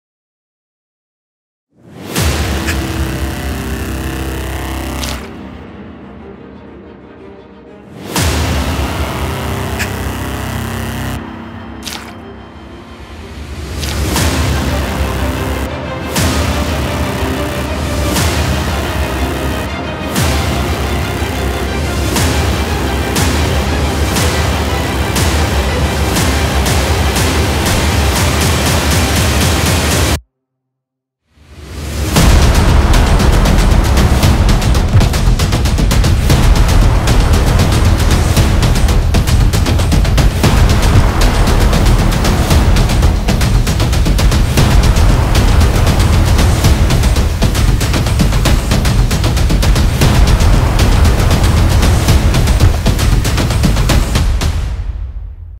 Showreel Events 2023 | Agenzia YES
Un recap dei maggiori eventi che abbiamo organizzato e realizzato durante questo 2023 appena concluso.. Buona visione!